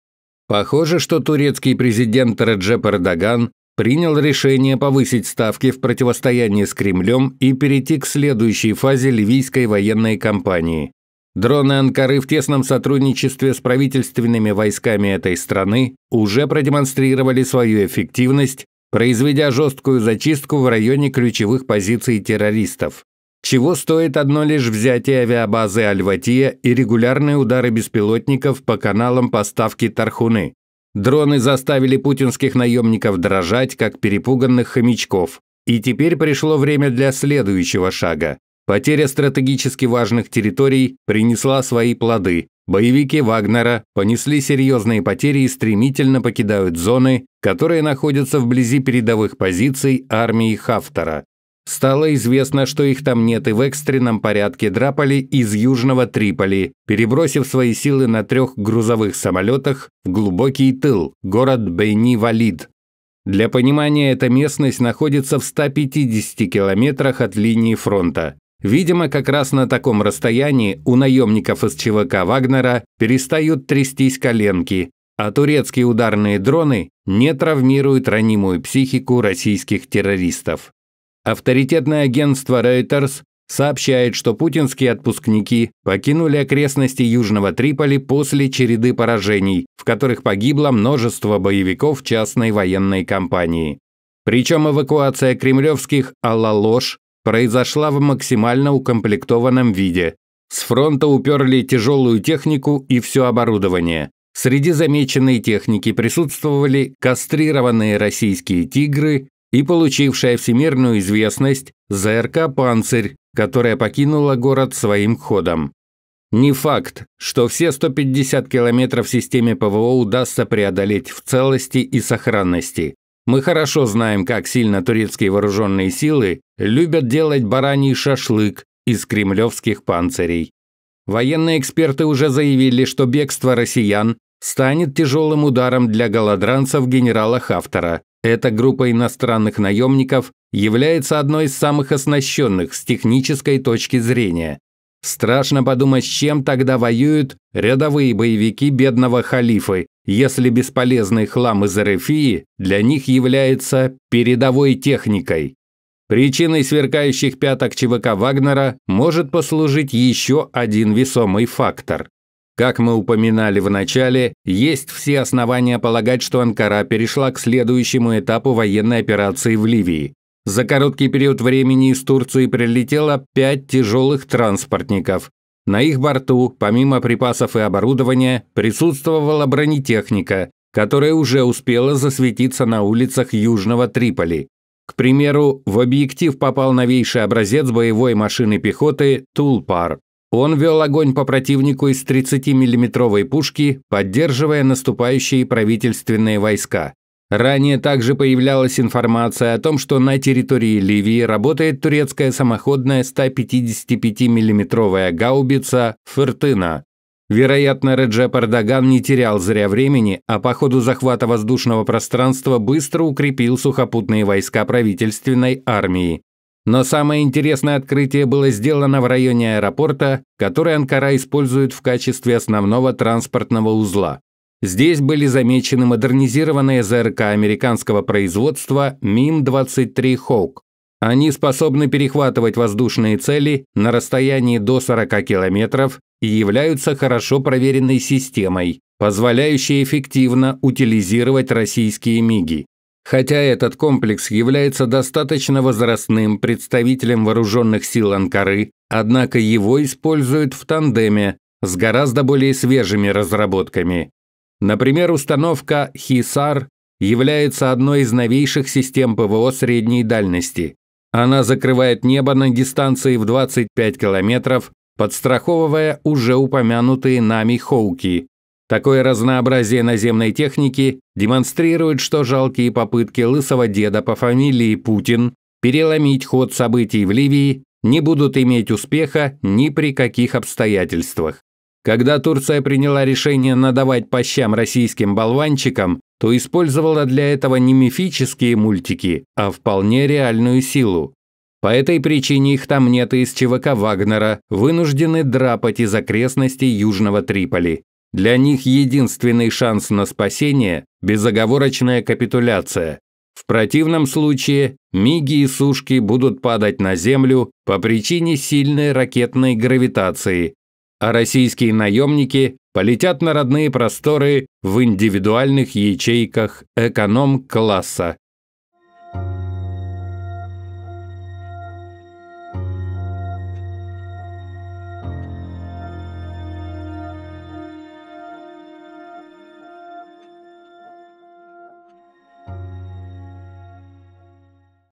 Подарок для Путина: Турция готовит стальной кулак
Подарки для Путина: Турция перебросила в Ливию новейшую бронетехнику и
системы ПВО
Автор: Павел Кот